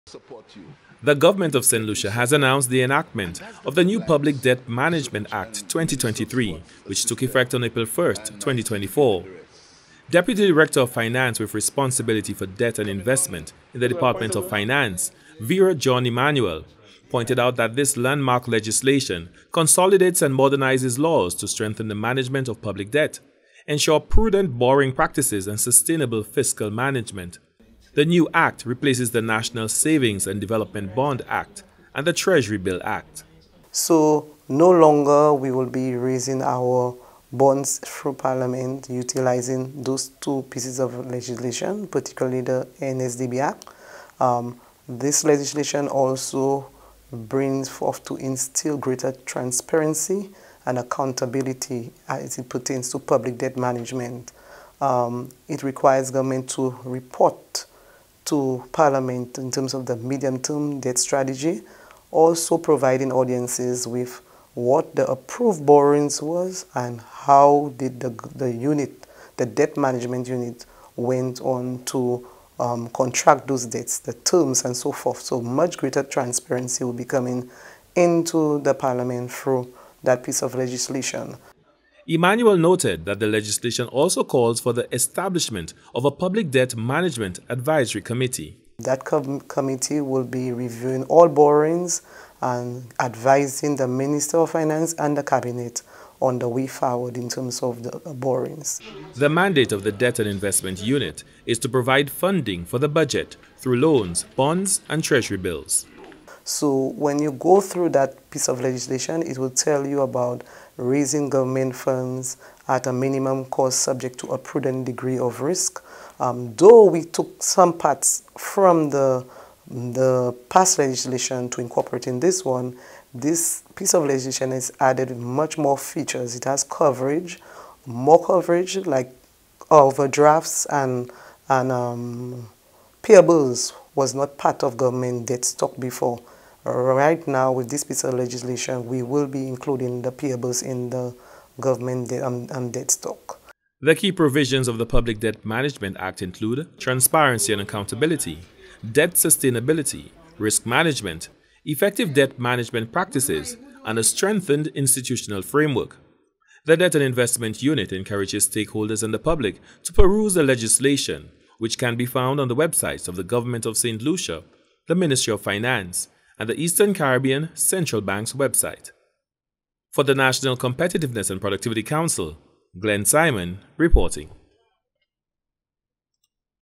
The Government of Saint Lucia enacts New Public Debt Management Legislation
The Debt and Investment Unit in the Department of Finance announced the enactment of the new Public Debt Management Act 2023.  The commencement of the Public Debt Management Act represents a significant step forward to ensure sound fiscal management and sustainable economic growth. This landmark legislation reaffirms the government’s commitment to responsible debt management practices and will contribute to the overall resilience of Saint Lucia's economy.